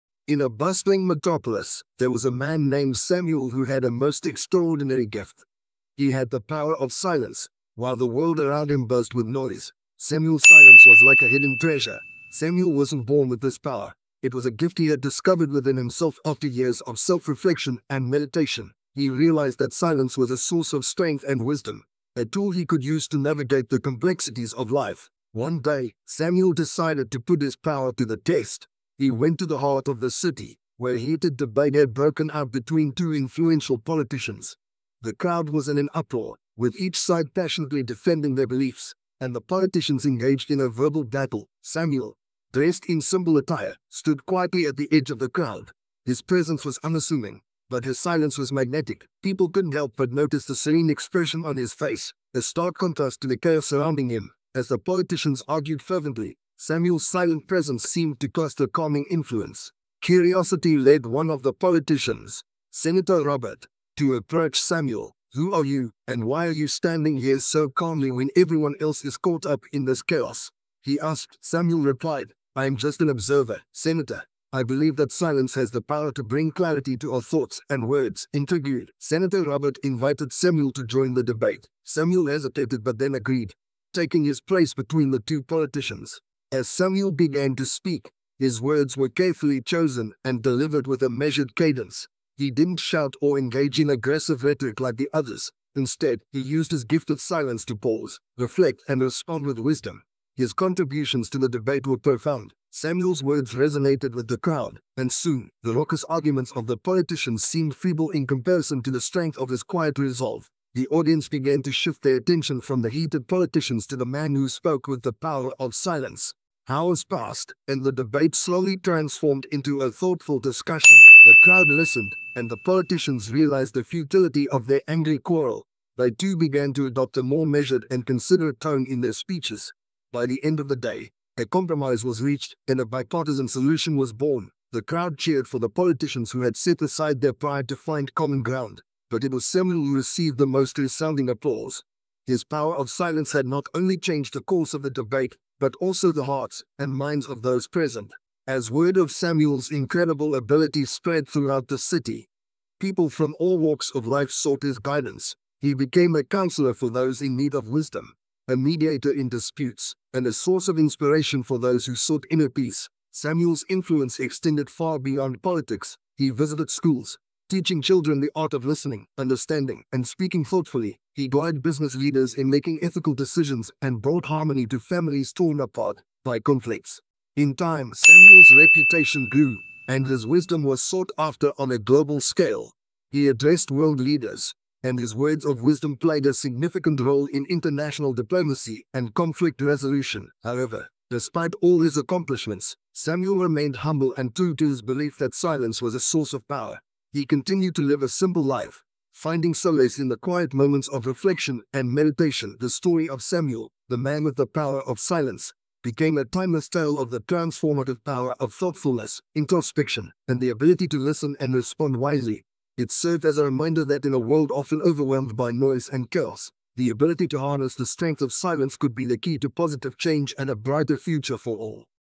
SILENCE IS POWER | ENGLISH STORY | SHORT STORY | BEDTIME STORY | I TELL TALES
This story teaches us how one can be valuable by being silent. We should all bear in mind that silence itself is an answer. Silence makes your words more impactful and heavy. People instantly pay attention to listen to your words.

We hope you like our story and support us to grow our channel. We post stories every Sunday and Wednesday.